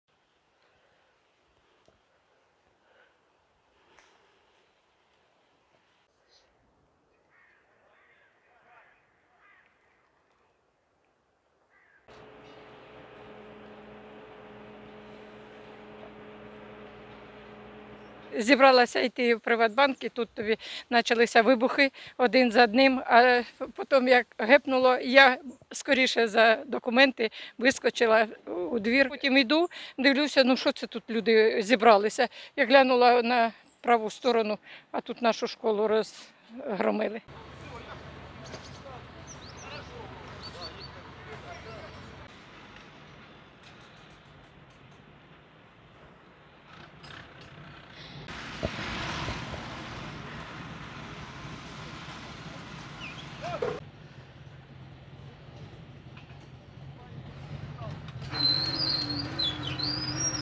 Війська РФ атакували Запоріжжя ракетами С-300 | Новини
Близько 08:30 ранку 21 жовтня в Запоріжжі пролунала серія вибухів. Російські військові здійснили атаку на обласний центр, застосовуючи ракети С-300. Загиблих немає, п'ятеро людей отримали поранення. Пошкоджена інфраструктура міста, зокрема школа.